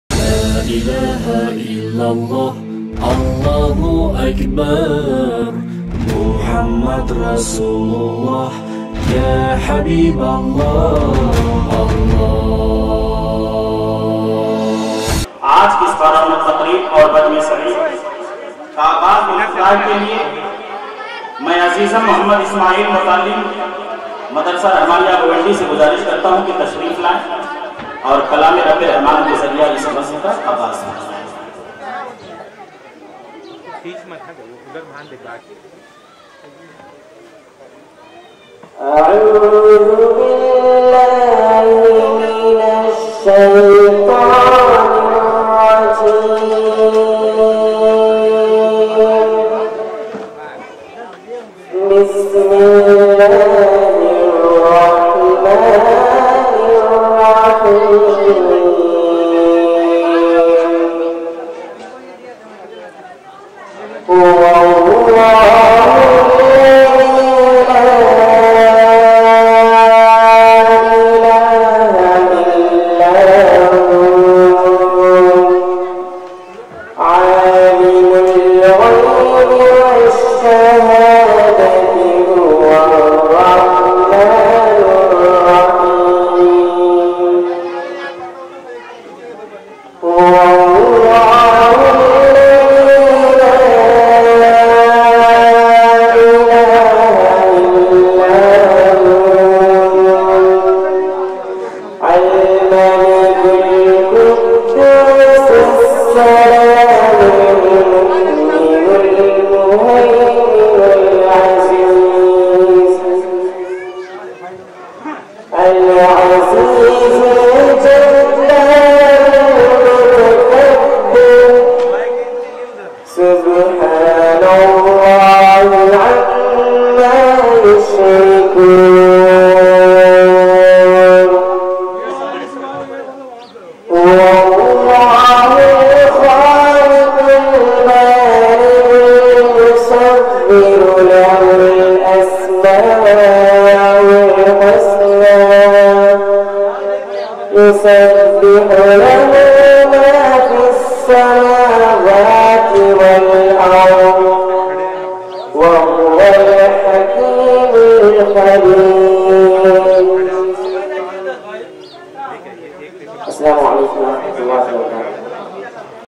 MOHD ISMAIL, TILAWAT, Mandala, govandi, jalsa, on 4th February 2018.
MOHD  ISMAIL, TILAWAT
siratun Nabi sallallahu alahe wassalam conference.
Held on 4th February at mandala, govandi, mumbai
Organised by - members of masjid ahle hadis and madarsa aljamiyatul khairiyatul salfiya.